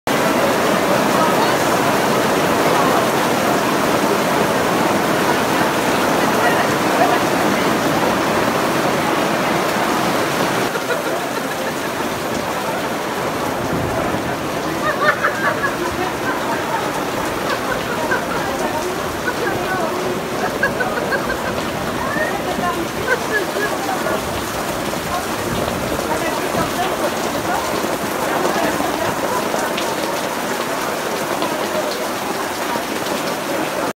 Josselin. L'orage gronde sur les Nocturnes Littéraires
Josselin, le 25 juillet 2021. Un violent orage s'est abattu sur la ville alors que les participants aux Nocturnes Littéraires venaient tout juste de s'installer...